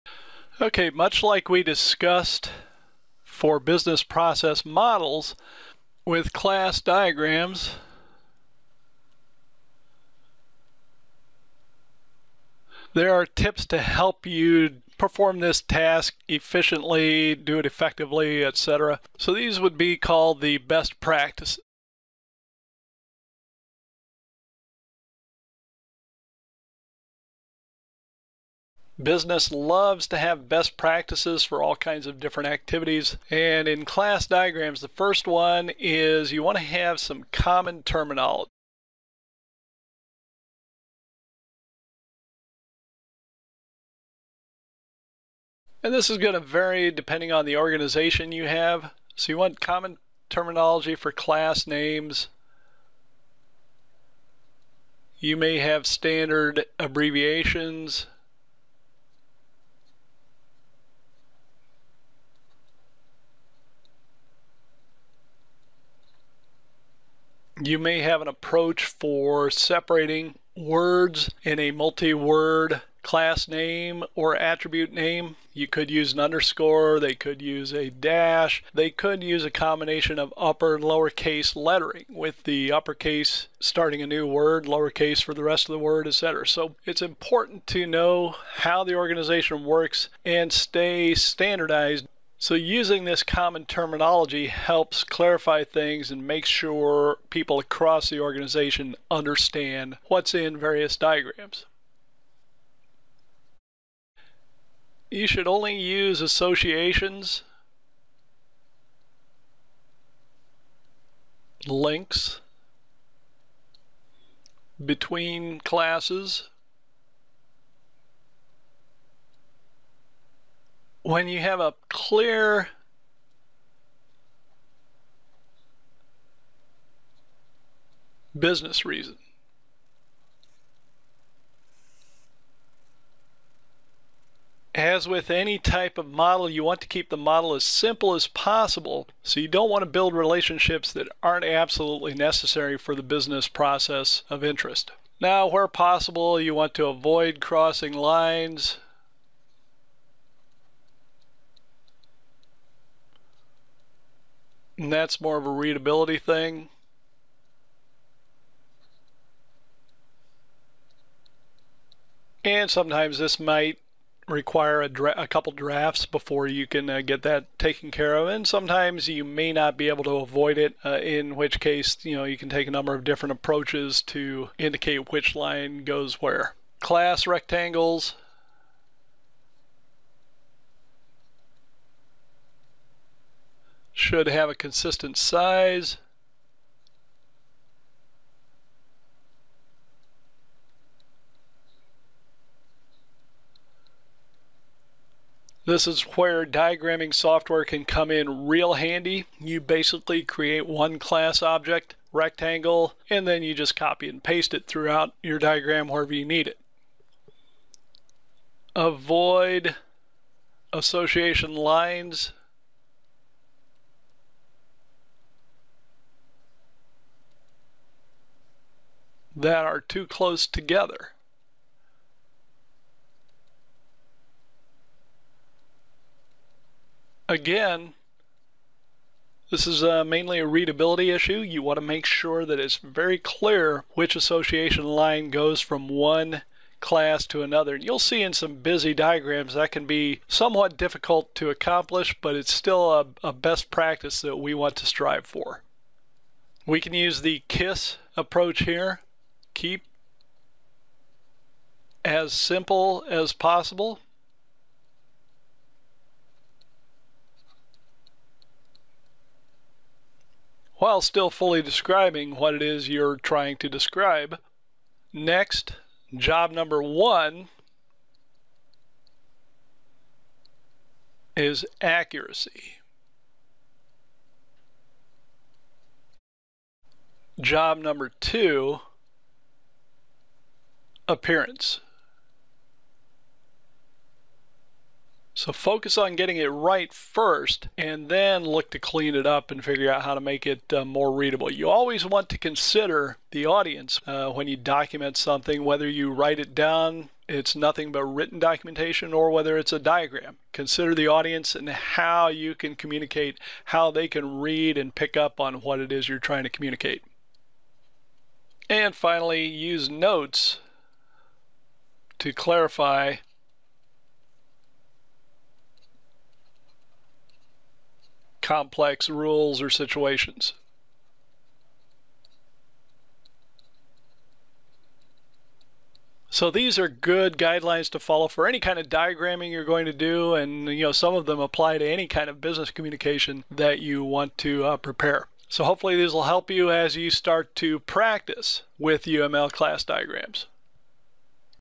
Class Diagram Best Practices
Best Practices when preparing Class Model diagrams (also applies to Entity Relationship Diagrams